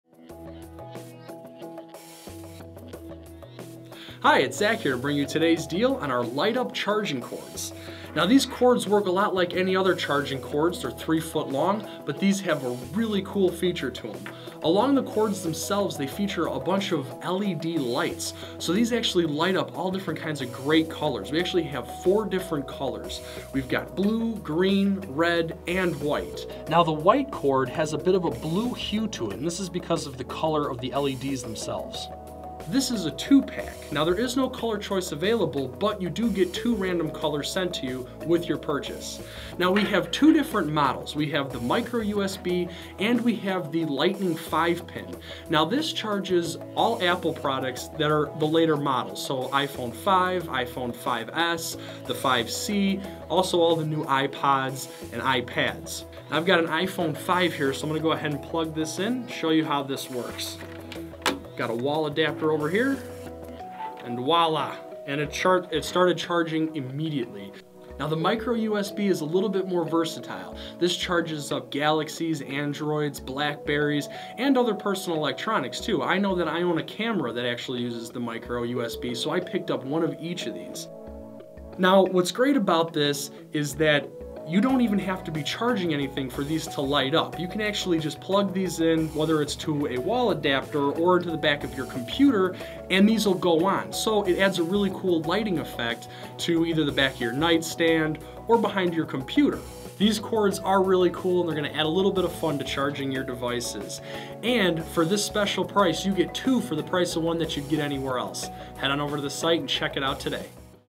LED Light Up USB Cords (2pk) Choose iPhone 5 or Micro USB
These USB Light Up Cords use a series of tiny LED lights to add a cool and unique glow effect. All you have to do is plug them into a powered USB slot, whether its in a computer or USB wall-adapter. You don't even have to be charging anything, so this doubles as night light or ambient backlight to your computer or side table.